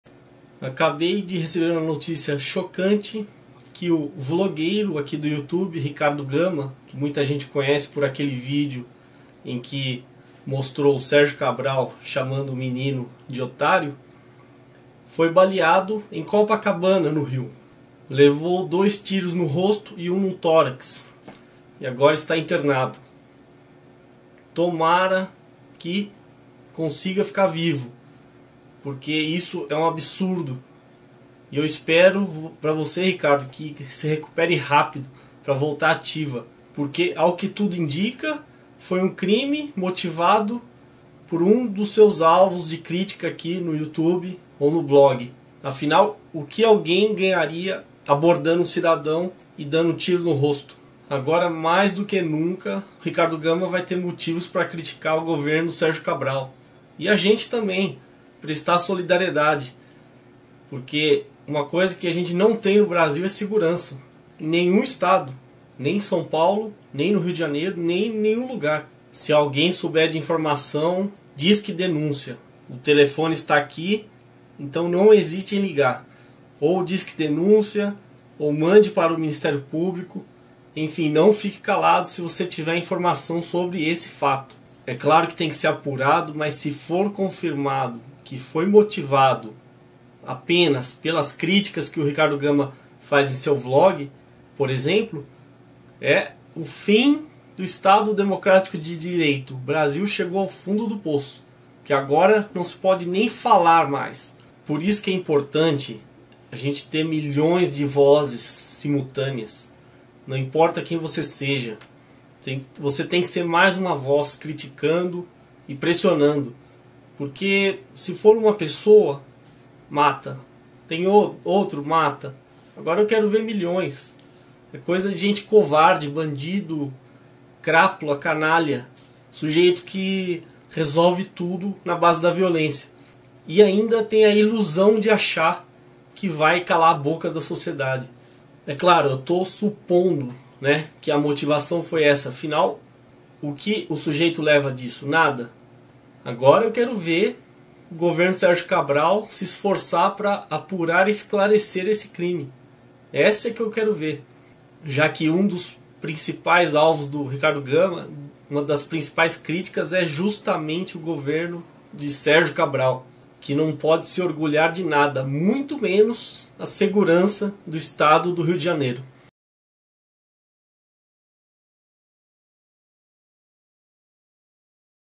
Quem tentou matar Ricardo Gama? Se souber, DENUNCIE!
Blogueiro baleado no Rio respira com ajuda de aparelhos, diz hospital
Segundo unidade, Ricardo Gama não corre risco de morte.
Ele foi atingido por dois tiros perto da Rua Santa Clara, em Copacabana.

O blogueiro Ricardo Gama, baleado na manhã desta quarta-feira (23), em Copacabana, na Zona Sul do Rio de Janeiro, está internado na UTI neurológica do Hospital Copa D'Or, no mesmo bairro. Segundo a direção do hospital, ele passou por uma cirurgia de quatro horas, e respira com auxílio de aparelhos. O seu estado de saúde é considerado estável.

Ricardo Gama foi atingido por dois tiros na cabeça e um no peito, perto da Rua Santa Clara. Ele foi socorrido por moradores da região. De acordo com o Copa D'Or, o blogueiro não corre risco de morte. Ainda não há previsão de alta do hospital.

A polícia tenta identificar o criminoso responsável pelos disparos. Segundo as investigações, os motivos do crime podem ter sido os artigos que a vítima escrevia numa página da internet.  Ricardo Gama mantém uma página onde posta vídeos, fotos e textos. Nos artigos, o blogueiro costuma fazer diversos tipos de críticas aos problemas da cidade.

Para a polícia, o crime pode ter relação com as informações que o blogueiro publica na internet. "Nós temos ali várias pessoas que poderiam ter se sentido ofendidas ou mesmo que poderiam ter como objetivo silenciar o que ele vinha escrevendo", disse o delegado Bruno Giladerte.

Peritos vão analisar imagens
O crime ocorreu na Travessa Moacyr Deriquem, que liga o Bairro Peixoto à Rua Santa Clara, um local considerado tranquilo em Copacabana. Os peritos vão analisar as imagens feitas pelas câmeras de segurança da rua para tentar identificar o criminoso.

Segundo testemunhas, os disparos foram feitos por um homem que passou num carro prata, no fim da manhã.

Lula preocupado com prejuízo eleitoral e Sérgio Cabral xingando menino de 17 anos (na época)